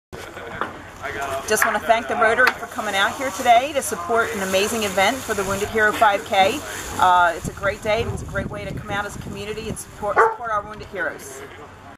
Rotary Supports Wounded Heroes 5K Run 2017
Event Chair Heather Sliwinsky thanks the Huntingdon Valley Churchville Rotary for their support of the 9/9/17 Run